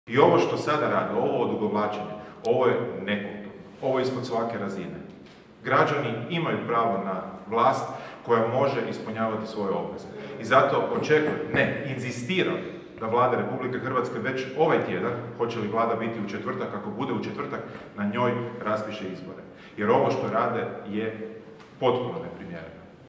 Grbin: "Očekujem da Vlada u četvrtak raspiše izbore"
Predsjednik SDP-a Peđa Grbin, potpredsjednica Sabina Glasovac, Matej Mostarac i dr.sc Neven Bosilj predstavili politiku stanovanja pod nazivom "Plan za stan".